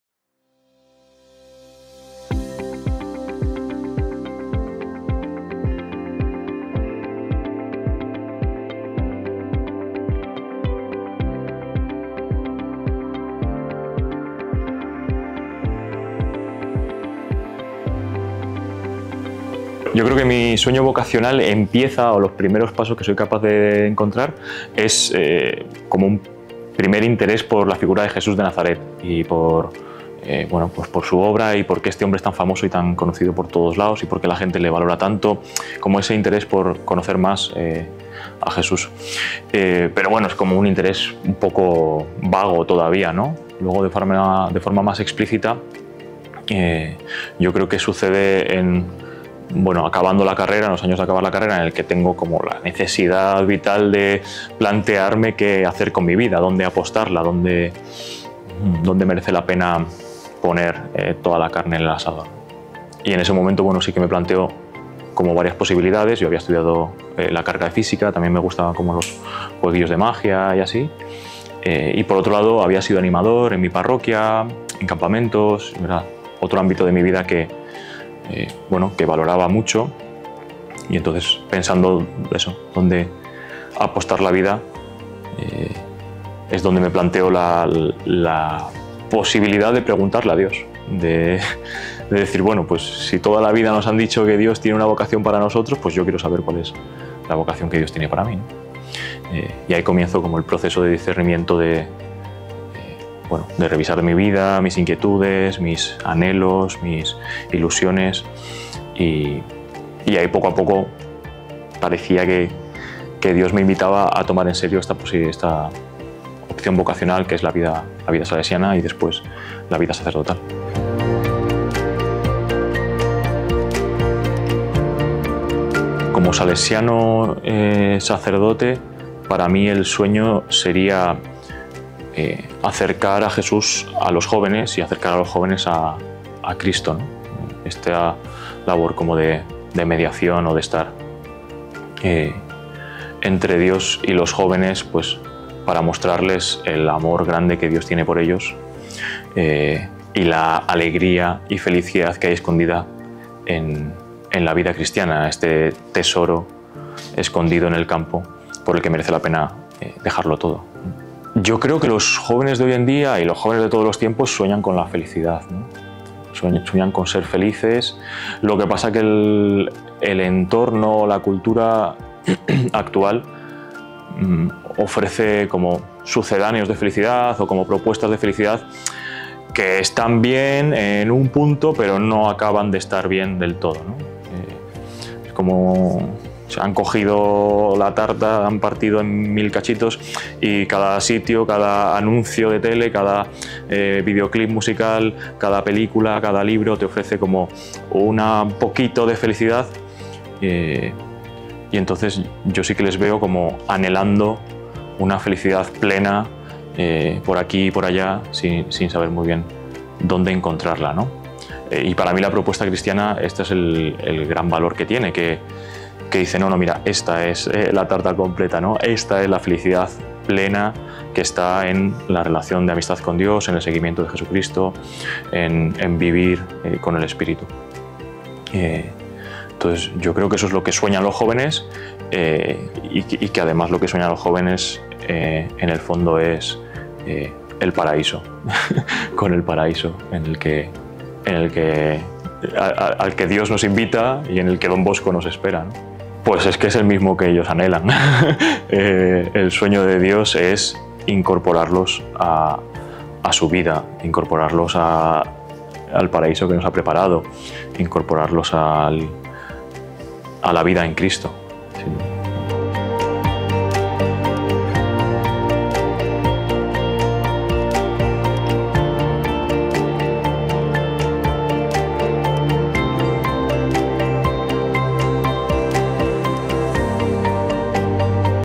Juan Carlos Aonso - Ordenaciones salesianas 2024
Juan Carlos Aonso es un joven salesiano que recibe el ministerio prebisteral el 8 de junio de 2024 en Salesianos Madrid-Atocha. En esta entrevista nos habla de sueños antes de dar tan importante paso en su vida.